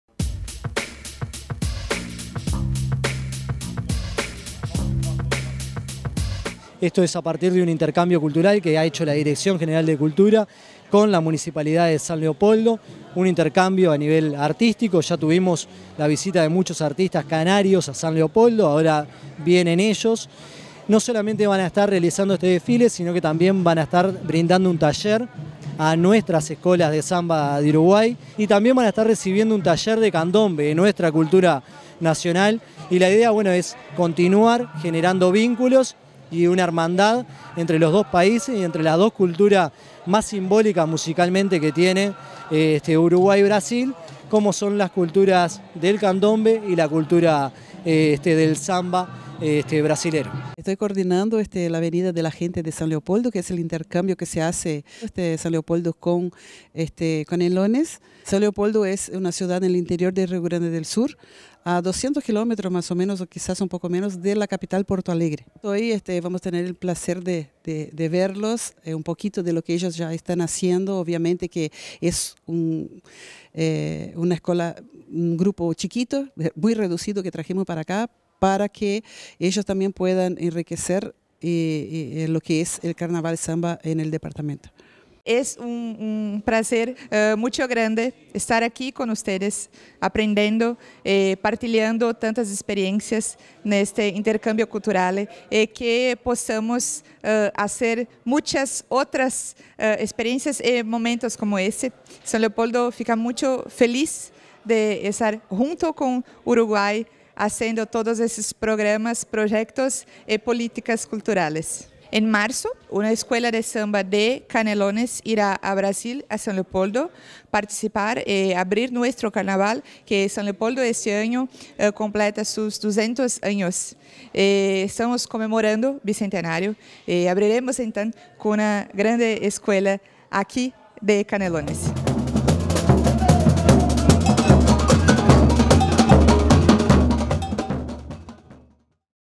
Escola de Samba Imperio Do Sol de Sao Leopoldo, Brasil, participó del desfile inaugural de Carnaval
En el marco de un convenio cultural que realizó la Dirección de Cultura de la Intendencia de Canelones con el Municipio de Sao Leopoldo, en Rio Grande do Sul, una escola de samba integrada por artistas de siete escuelas brasileñas visitó el departamento. En esta línea, más de 30 artistas brasileños cerraron el desfile inaugural en la ciudad de Canelones y participaron de un taller de intercambio de saberes sobre samba y candombe en el parque Artigas de Las Piedras.